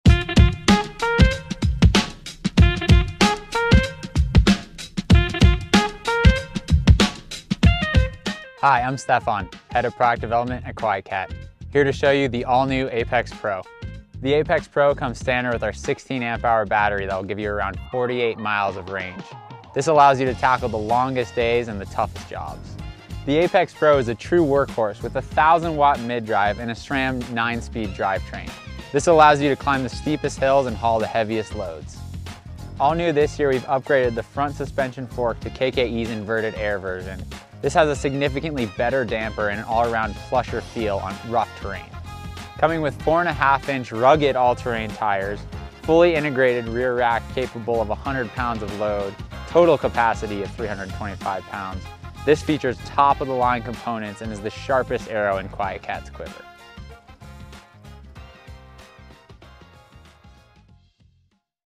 QuietKat Apex Pro All Terrain E Bike from Nomadic Supply Company
Discover unparalleled power, reliability, and carrying capacity with the QuietKat Apex Pro All-Terrain E-Bike from Nomadic Supply Company, designed for hunters, anglers, military personnel, law enforcement agencies, and businesses demanding top performance and adaptability.

Powered by a robust 1000W motor, the QuietKat Apex Pro All-Terrain E-Bike delivers unmatched torque and acceleration, allowing you to effortlessly overcome steep inclines, muddy trails, and tough terrain. This All-Terrain Electric Bike is engineered for backcountry exploration and will take you wherever your adventure leads.

Experience a smooth, comfortable ride on the most challenging landscapes with the QuietKat Apex Pro All-Terrain E-Bike's high-performance inverted front suspension fork, ensuring exceptional shock absorption and precise handling.

Confidently navigate any surface with the QuietKat Apex Pro All-Terrain E-Bike's all-terrain fat tires, providing exceptional traction, stability, and control on rocky paths, slippery slopes, and beyond.

With its integrated heavy-duty pannier rack, the QuietKat Apex Pro All-Terrain E-Bike accommodates all your gear, from hunting equipment to work supplies, offering an industry-leading 100-lb hauling capacity.

Customize your QuietKat Apex Pro All-Terrain E-Bike with a variety of turnkey accessories, including custom trailers, storage solutions, additional lighting, and weapon holders, tailored to meet your specific needs.